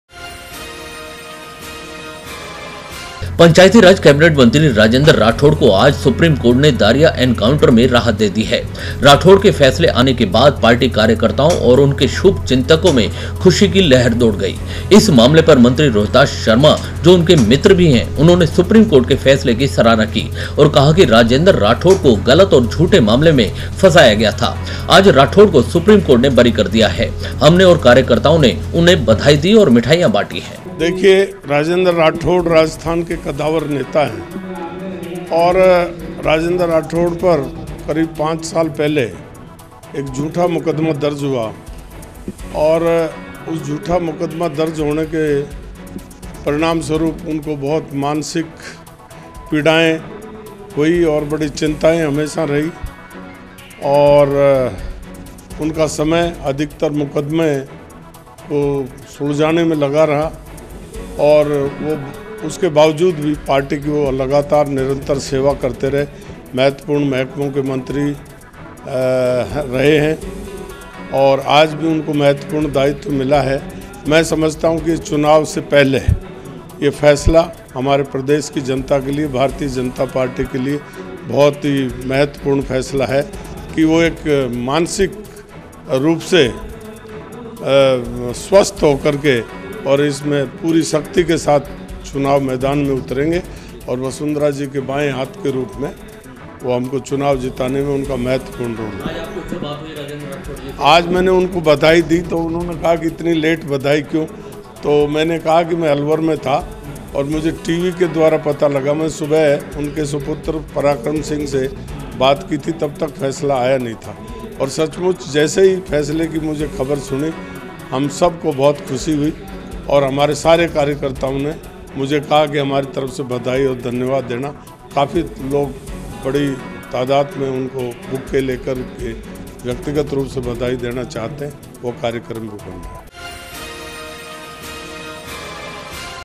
कैबिनेट मंत्री राजेन्द्र राठौड़ को सुप्रीम कोर्ट से मिली राहत, दारिया एनकाउंटर में चल रहा था मामला
-पंचायती राज केबिनेट मंत्री राजेन्द्र राठौड़ को आज सुप्रीम कोर्ट ने दारिया एनकाउंटर में राहत दे दी है। राठौर के फैसले आने बाद पार्टी कार्यकर्ताओं और उनके शुभचिंतकों में खुशी की लहर दौड़ गयी। इस मामले पर मंत्री रोहिताश्व शर्मा जो उनके मित्र भी है। उन्होंने सुप्रीम कोर्ट के फैसले की सराहना की ओर कहा राजेन्द्र राठौड़ को गलत ओर झूठे मामले में फंसाया गया था। आज राठौर को सुप्रीम कोर्ट ने बरी कर दिया है। हमने ओर कार्यकर्ताओ ने उन्हें बधाई दी और मिठाई बांटी।